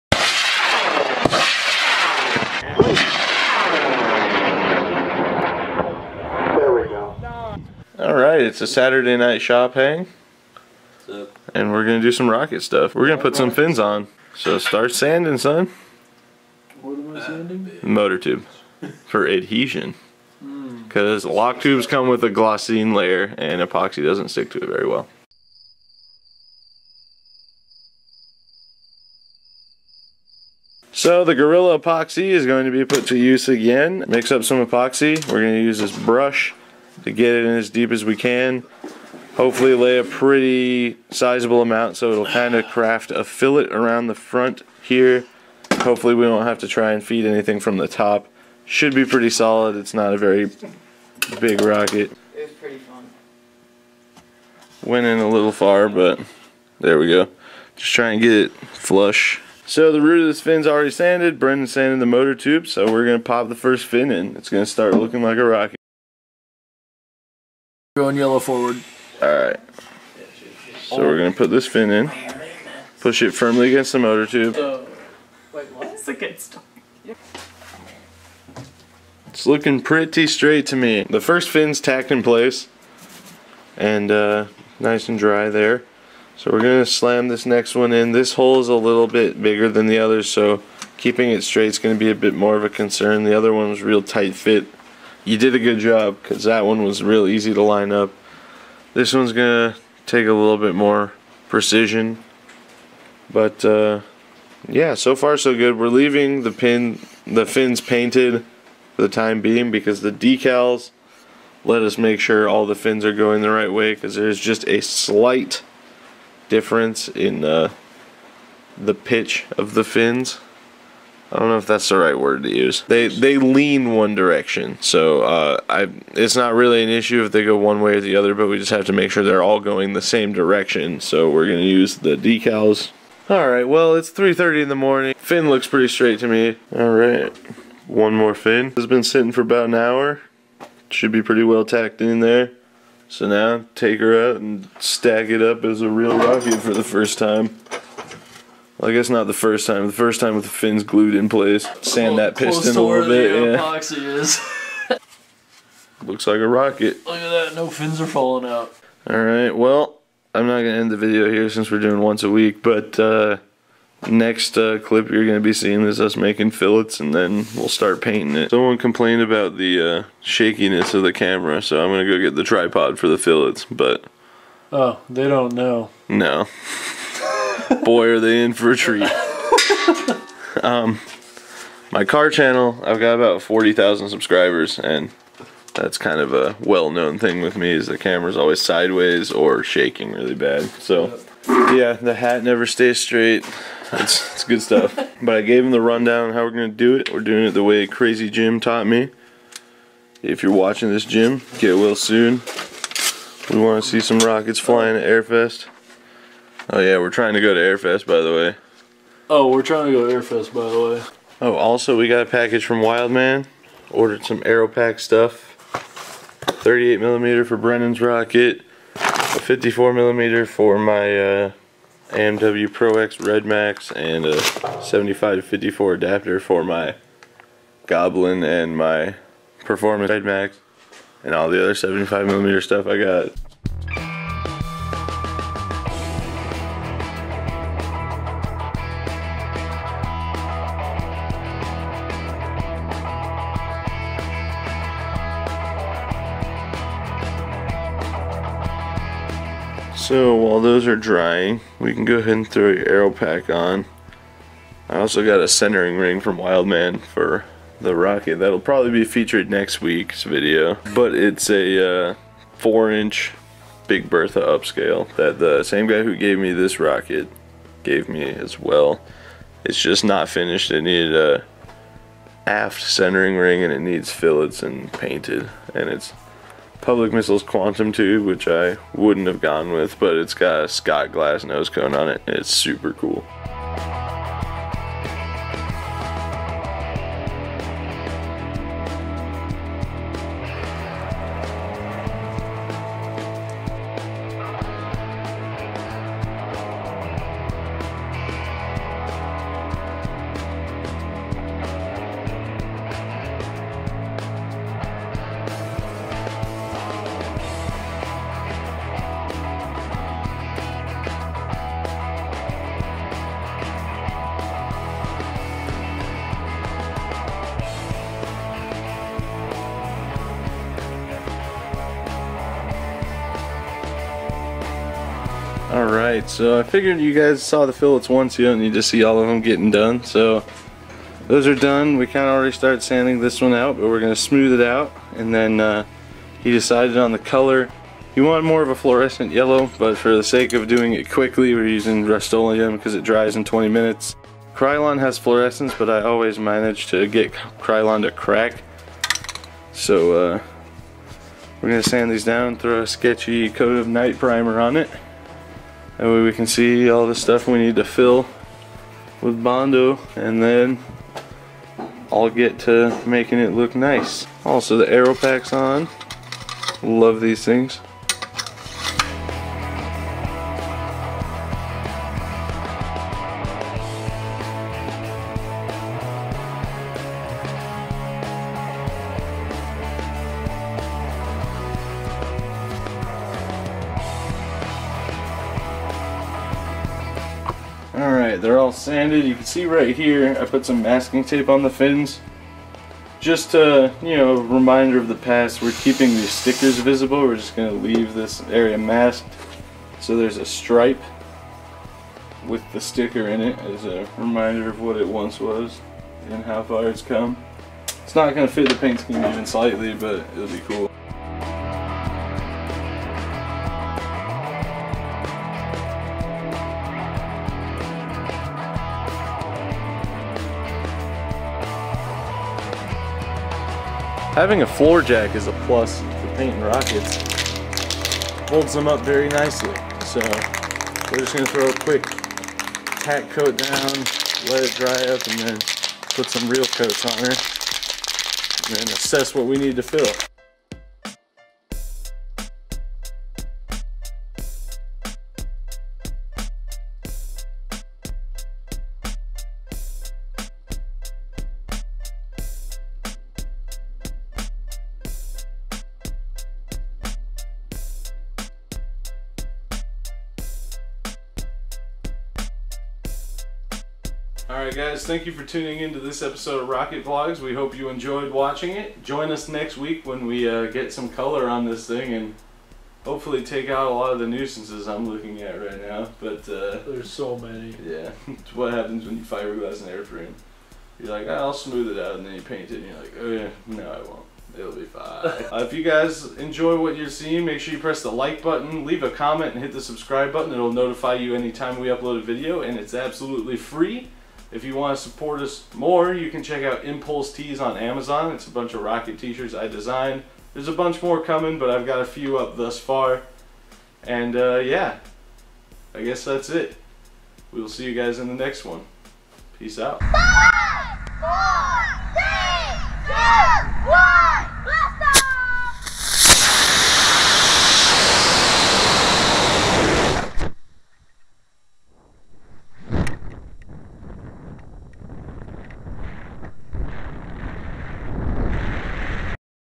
Building A High Power Rocket Part 2: Fillets, Final Assembly, Paint
Welcome to RocketVlogs! My room mate has been wanting to step into High Power Rocketry, and in this episode, we finish building the rocket, including motor and fin assembly, then finishing the fins off with fillets.